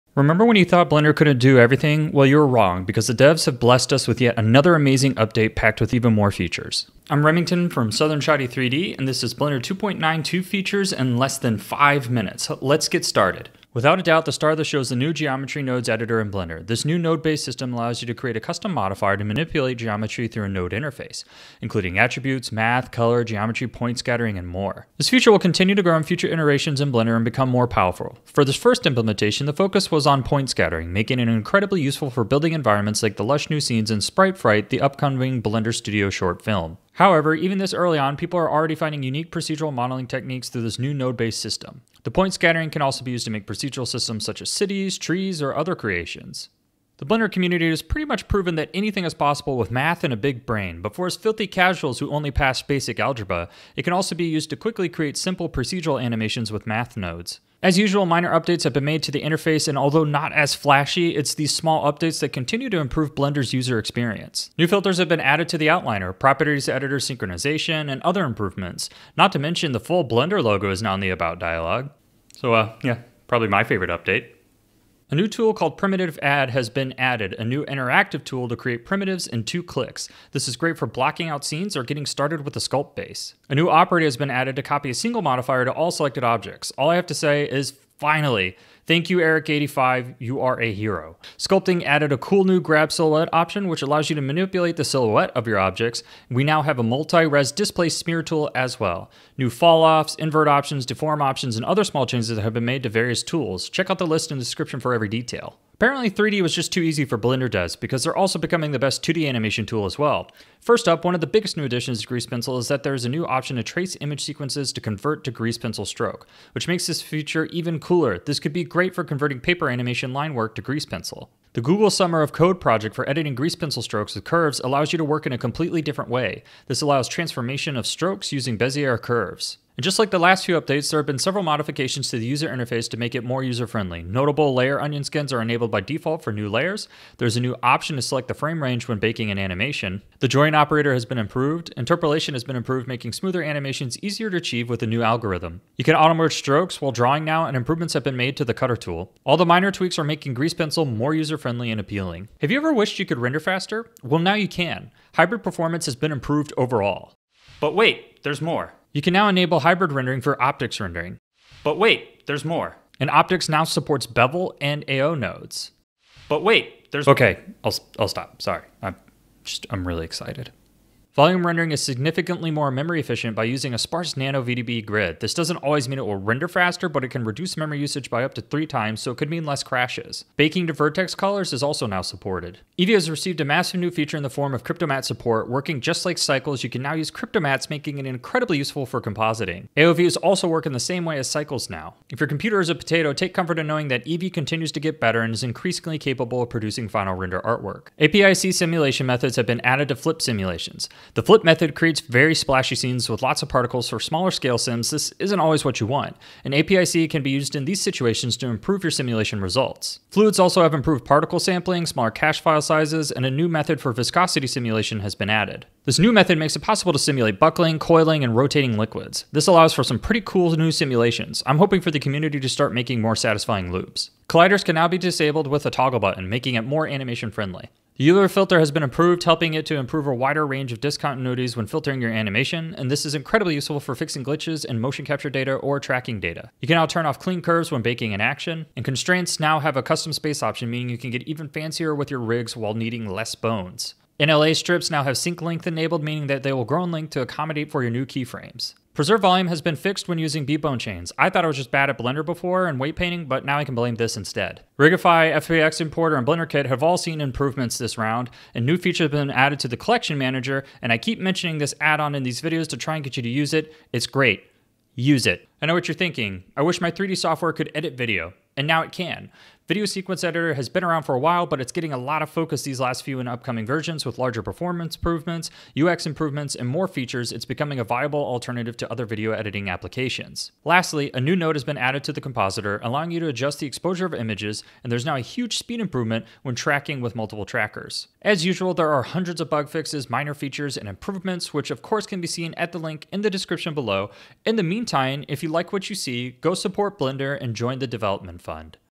Blender 2.92 New Features in LESS than 5 Minutes
Discover what's new in Blender 2.92 with @SouthernShotty!